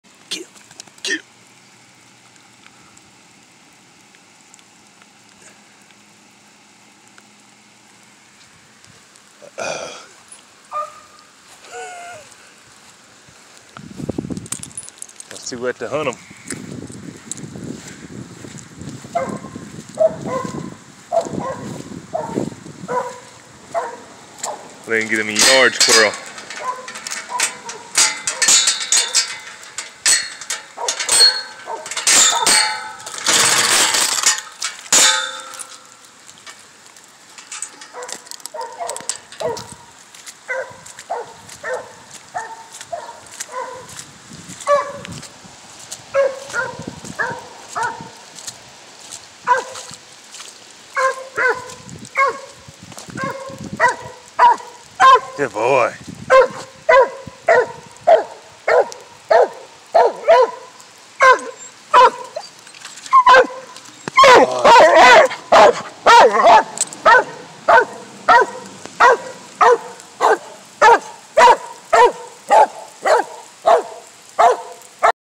When its too wet to hunt. He trees yard squirrels.
original mountain cur treeing squirrels.